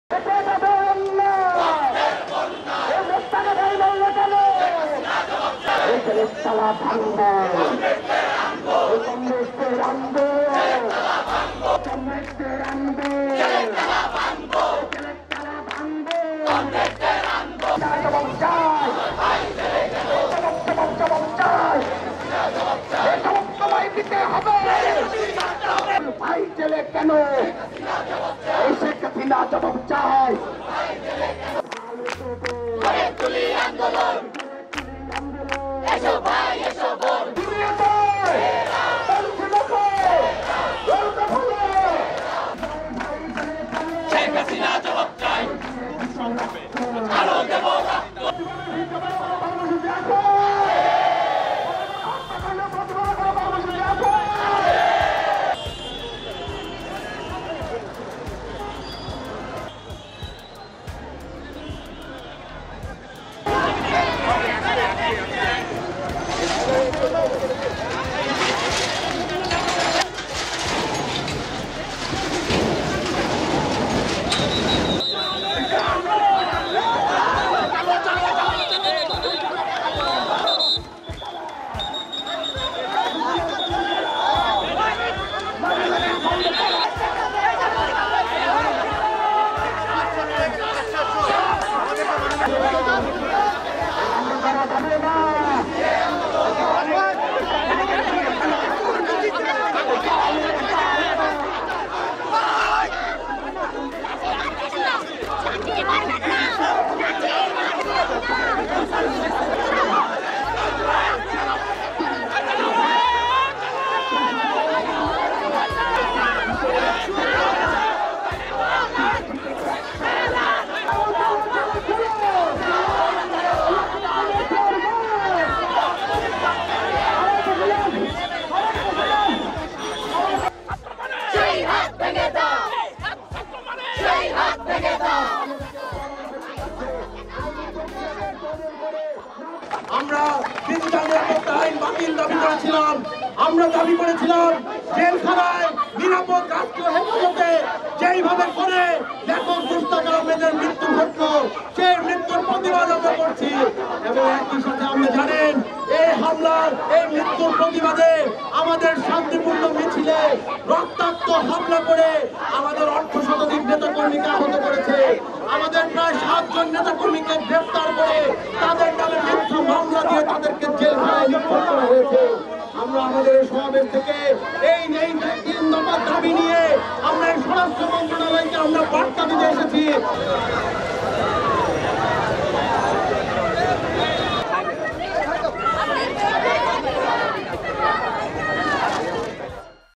Protests over Mushtaq’s death in jail continue
Progressive Students’ Alliance on Monday demonstrated near the Bangladesh Secretariat in the capital as the police stopped the procession from proceeding further.
The student alliance had a plan to lay siege to the home ministry housed in the Secretariat demanding justice for writer Mushtaq Ahmed who was detained under Digital Security Act and died in jail on February 25.
The left-leaning student combine has been agitating for days also for scrapping the controversial Digital Security Act, an immediate release of the detained protesters in Dhaka and Khulna and those arrested under the Digital Security Act, including cartoonist Ahmed Kabir Kishore.
It was the fourth consecutive day’s protests over the death of Mushtaq who had been detained for about nine months and denied bail six times.
Cartoonist Kishore, also a co-accused in the case, has been still in jail also since his arrest in early May 2020.
At around Monday noon, several hundred leaders and activists of Progressive Students Alliance carrying banners, placards and posters reading the demands began their scheduled march towards the home ministry from the Raju Memorial Sculpture on the Dhaka University campus chanting slogans for their demands and slamming the government.